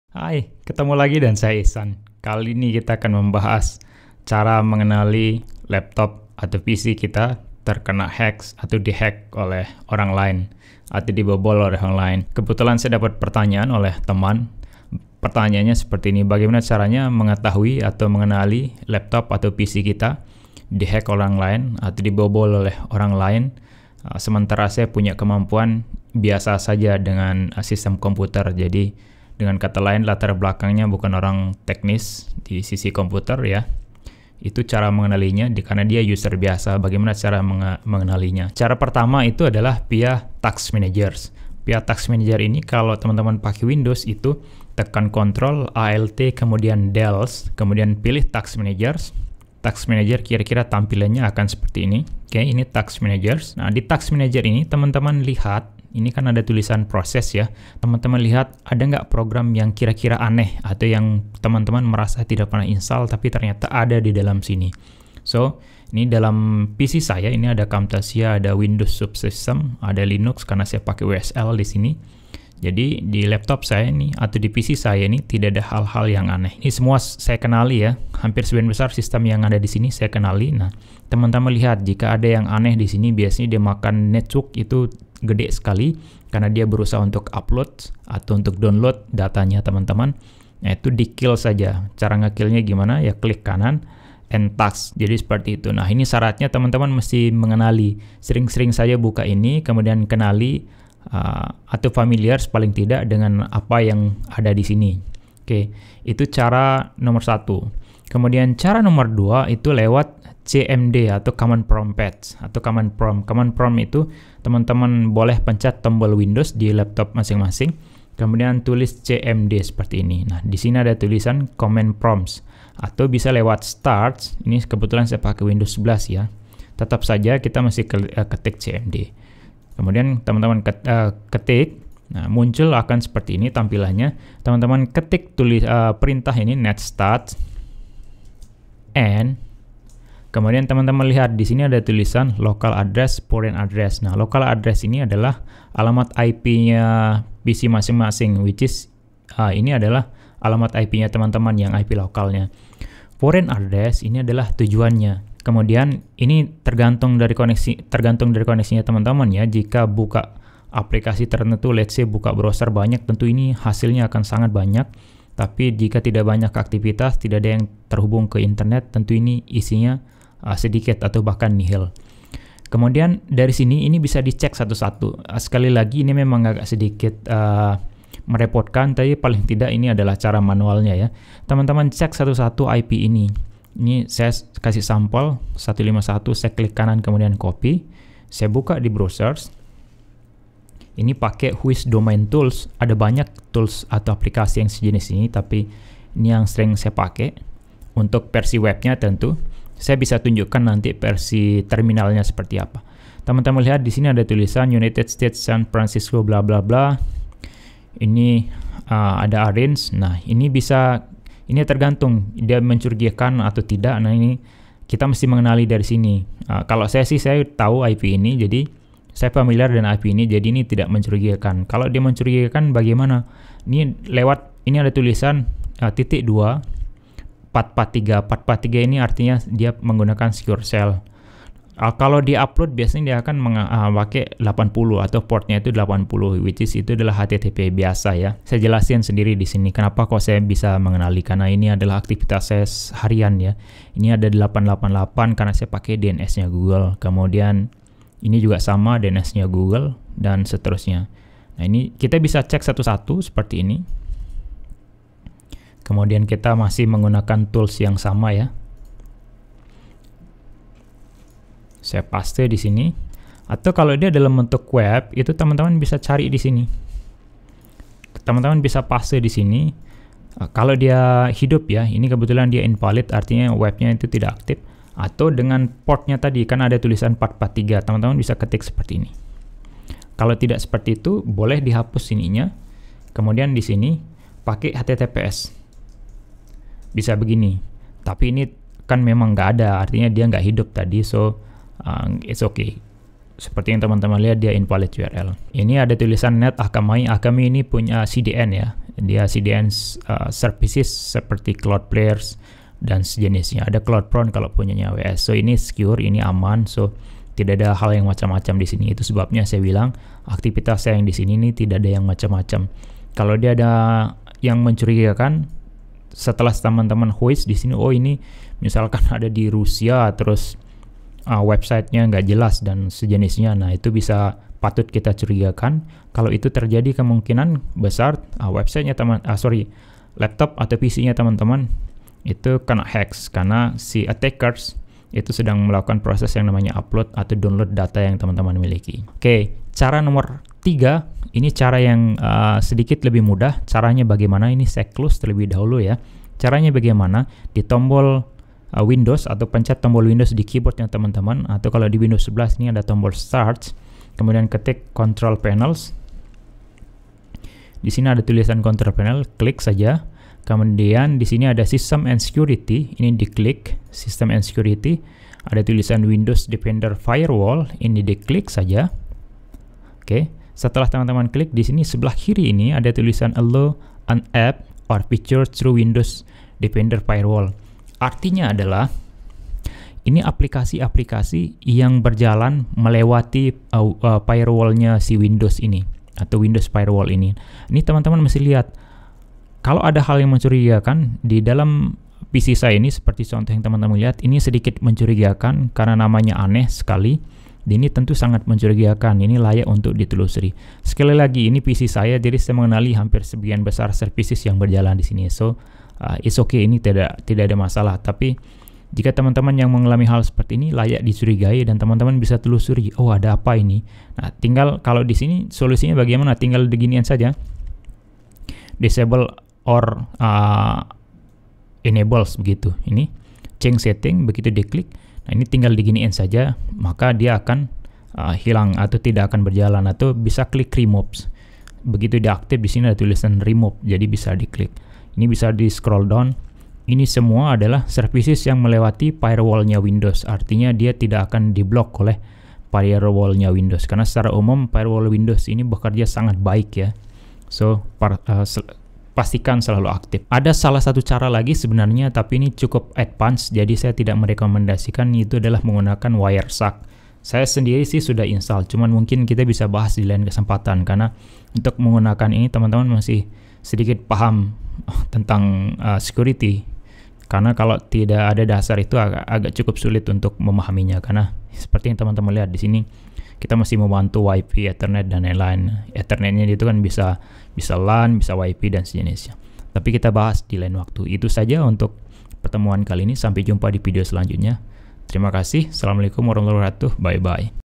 Cara Mudah Mengetahui Computer Anda di Hack
Video ini menjelaskan cara termudah untuk mengenali komputer anda di hack bahkan untuk orang yang tidak begitu mengerti cara kerja komputer.